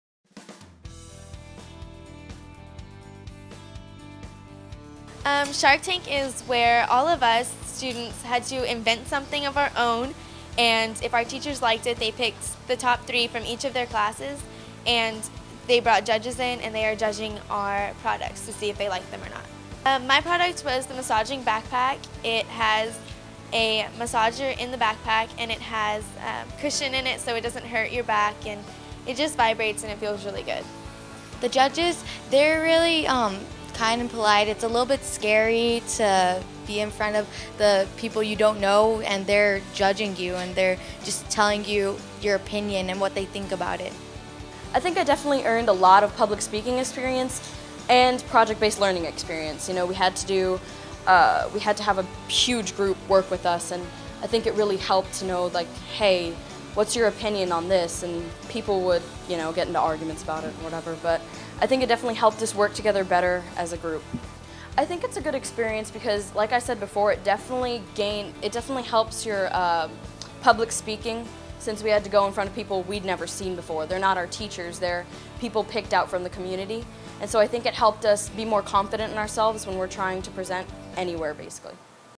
Coakley Middle School Shark Tank
8th graders at Coakley MS participated in a Shark Tank competition for their science class by inventing their own products and presenting them to a panel of judges.